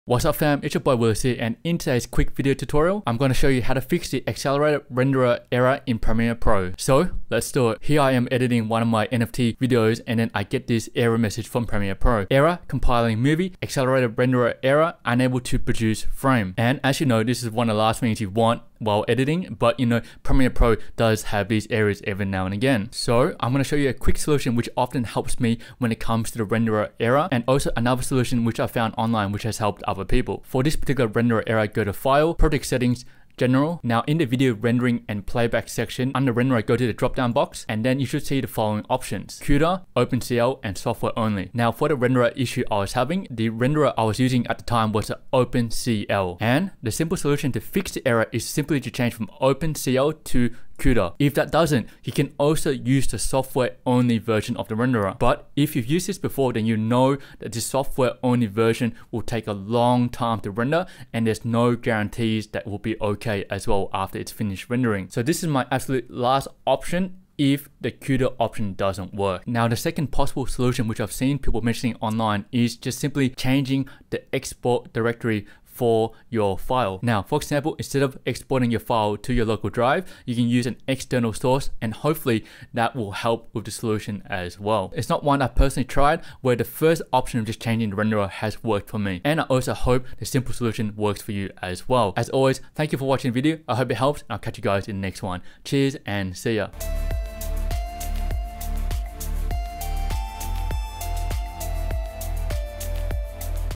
How to fix Accelerated Renderer Error in Adobe Premiere Pro
Hey fam, in this video tutorial I show you how to fix the Accelerated Renderer Error in Adobe Premiere Pro, so let's do it!